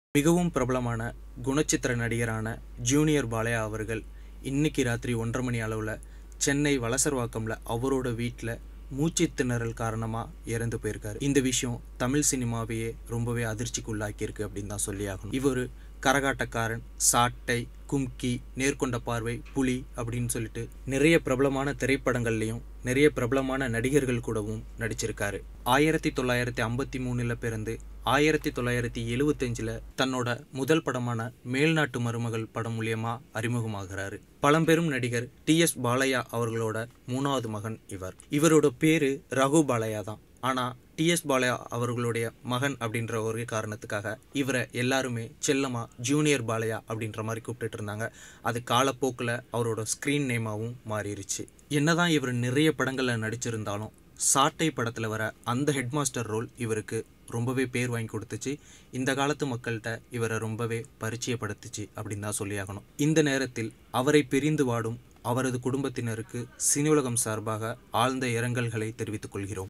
Actor Junior Balaiah Passed Away😱 நடிகர் ஜூனியர் பாலையா மரணம் | RIP Junior Balaiah
Actor Junior Balaiah Passed Away😱 நடிகர் ஜூனியர் பாலையா மரணம் | RIP Junior Balaiah | Cineulagam

This video is about the shocking news on Veteran Tamil Actor Raghu Balaiah Aka Junior Balaiah passing away earlier today due to suffocation at his Chennai Residence. Cineulagam extends its heartfelt condolences to his Family & Friends.